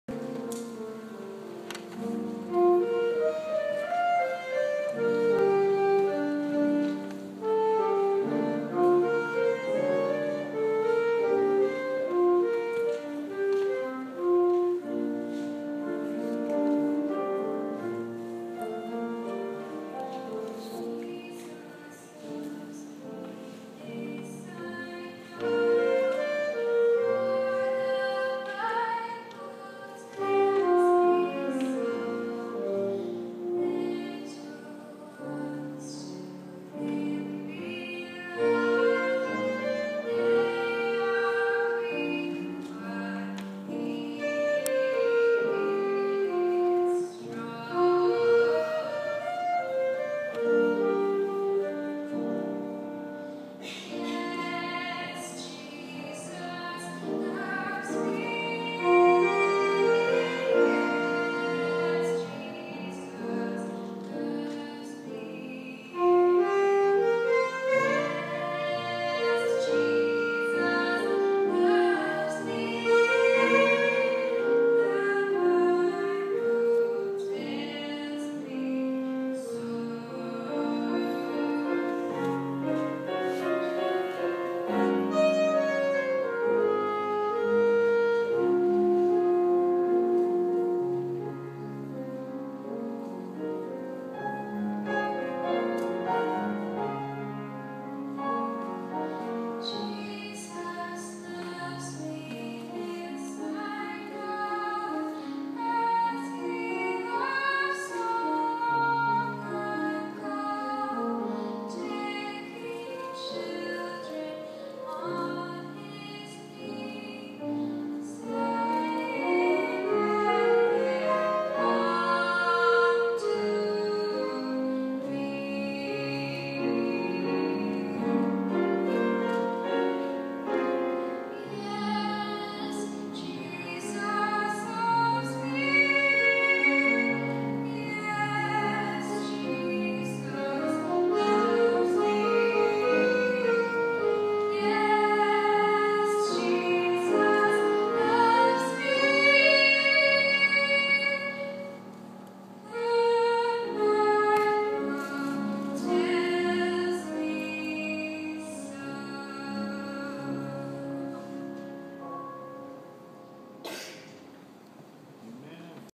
Youth Sunday 2013
Nick Kettler and Caroline Hersman perform on Youth Sunday at CCPC